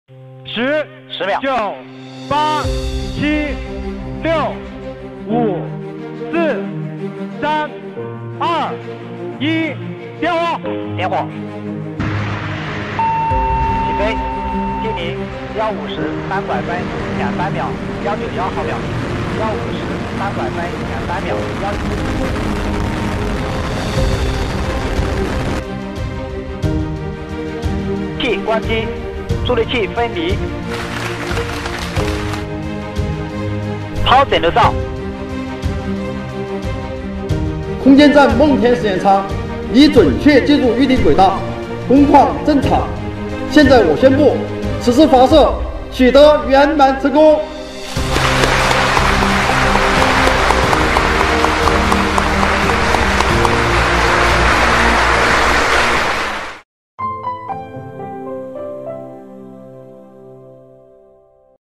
1分钟回看梦天实验舱发射全过程
北京时间10月31日15时37分，搭载空间站梦天实验舱的长征五号B遥四运载火箭，在中国文昌航天发射场准时点火发射，约8分钟后，梦天实验舱与火箭成功分离并准确进入预定轨道，发射任务取得圆满成功。